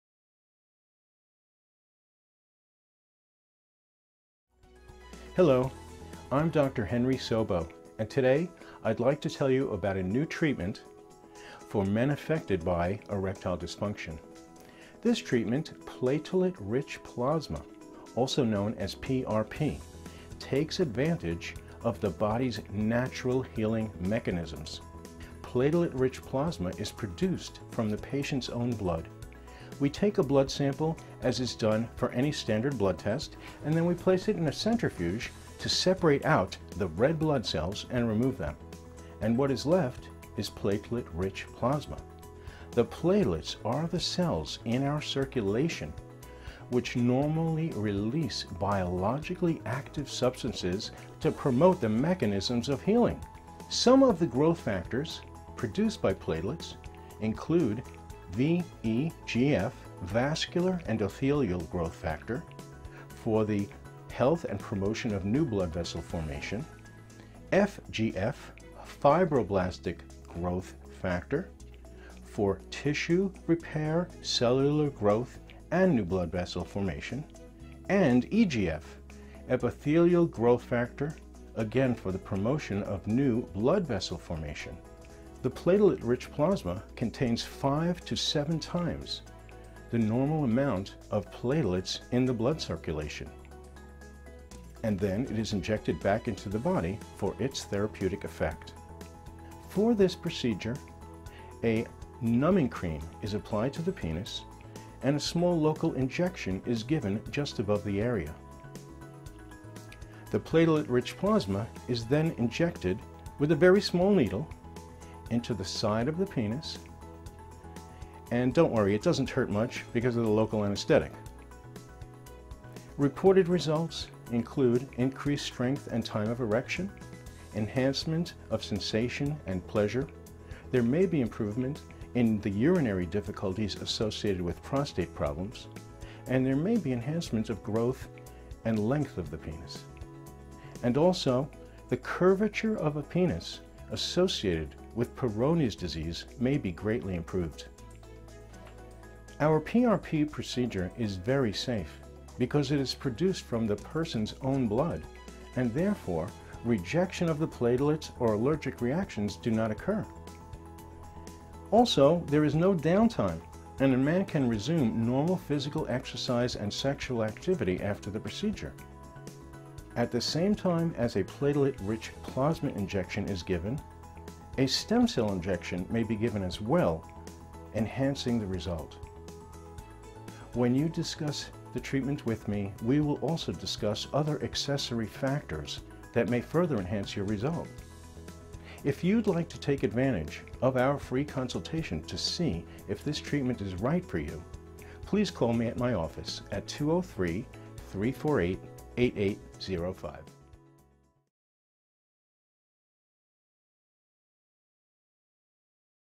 PRP and Stem Cells for Erectile Dysfunction
Dr. Henry Sobo discusses a new treatment for Erectile Dysfunction (ED) using PRP and stem cells.